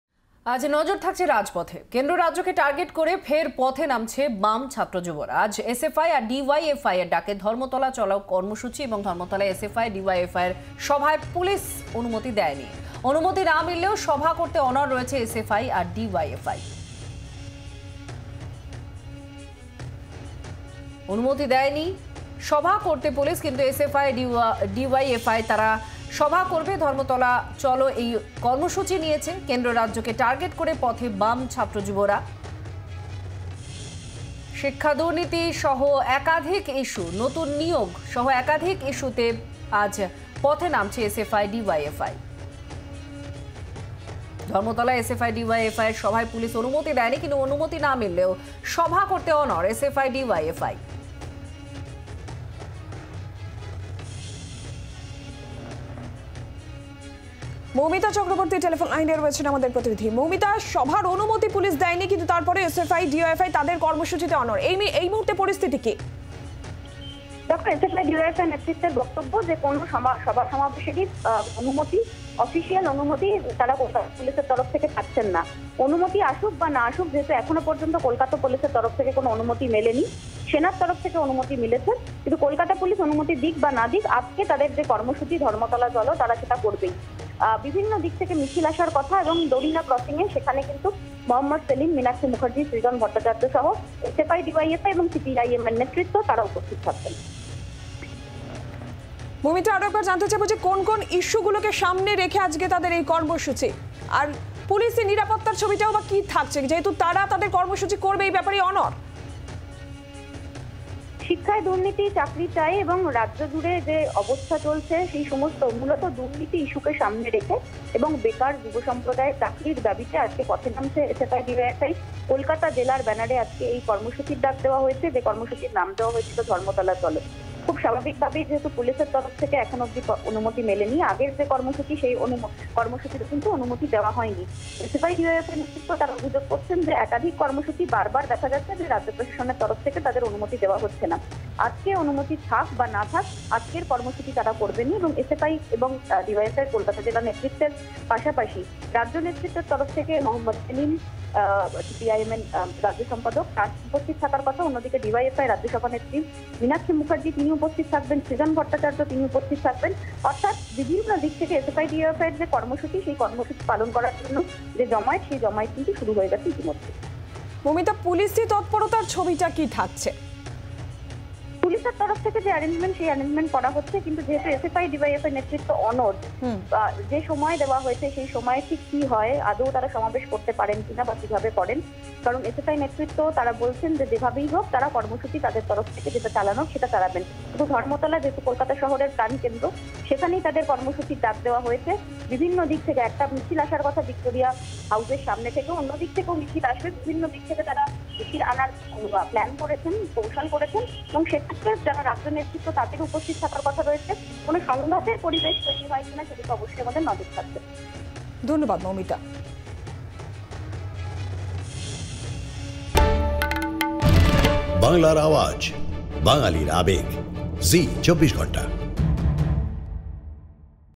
SFI Rally: 'ধর্মতলা চলো' কর্মসূচিতে অনুমতি না মিললেও সভা করতে অনড় SFI, DYFI | Zee 24 Ghanta
SFI Rally: 'ধর্মতলা চলো' কর্মসূচিতে অনুমতি না মিললেও সভা করতে অনড় SFI, DYFI...